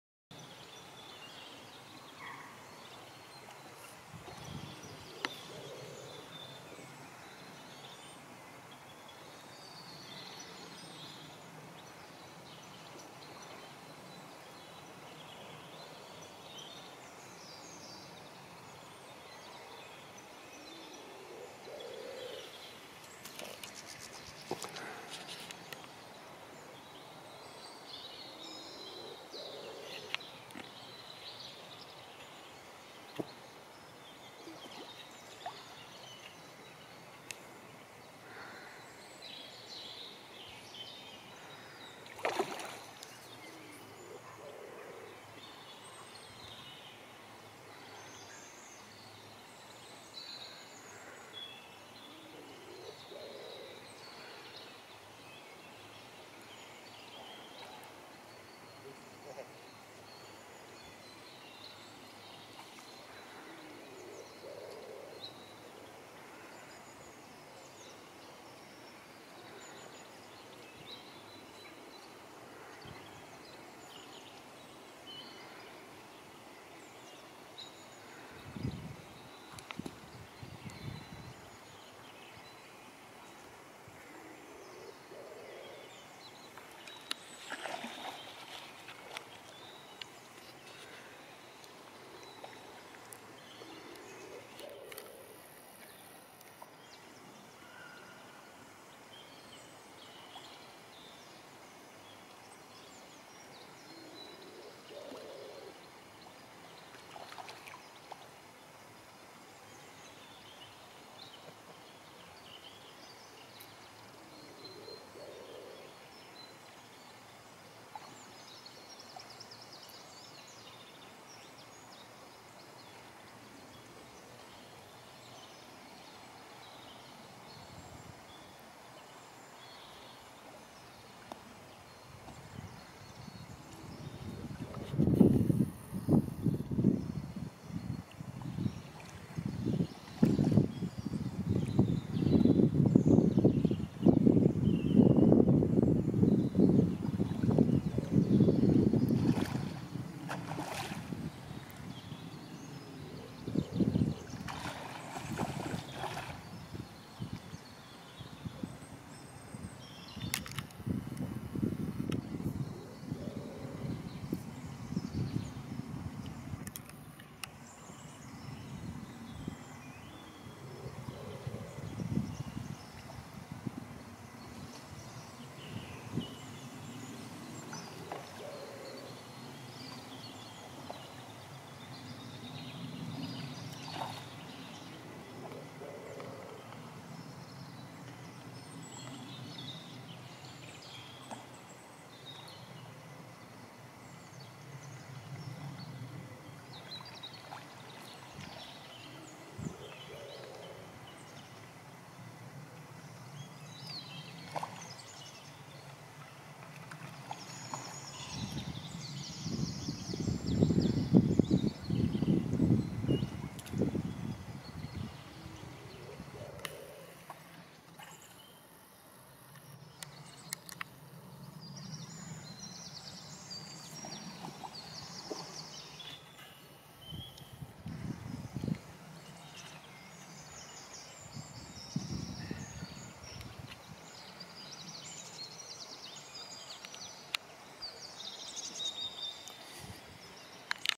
Fly fishing for big trout
Sometimes I’m drawn inland away for the salty stuff … trout fishing is great sport ✌🏻